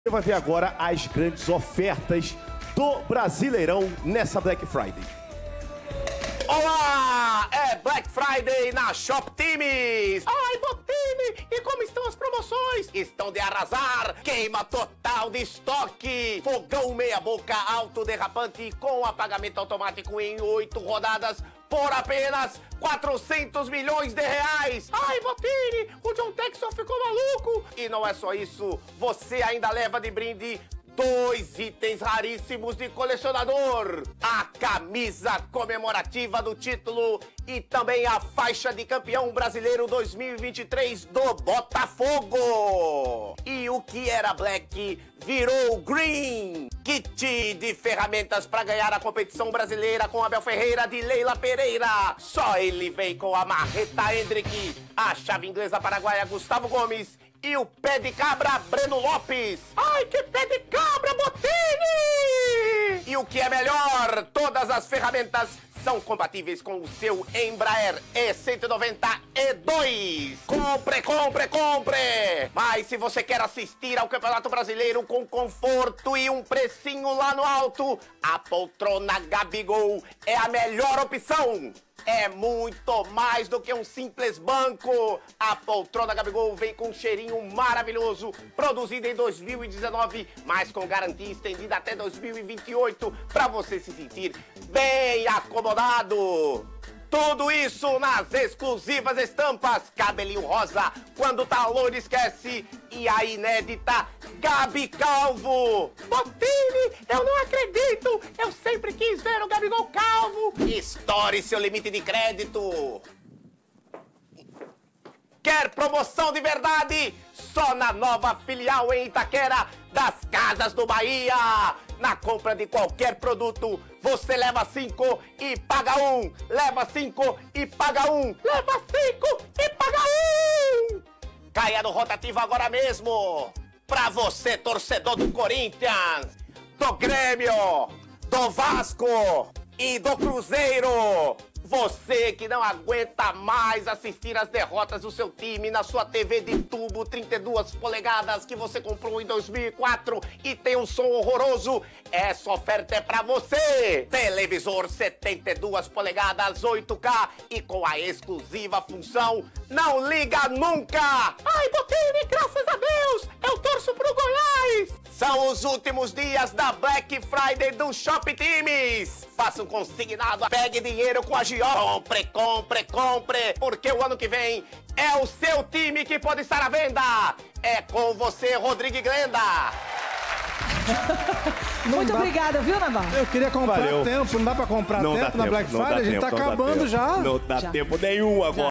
Black Friday shop-times! Confira os descontos imperdíveis do Brasileirão
Navarro mostra com muito humor o que está acontecendo no Brasileirão nos último dias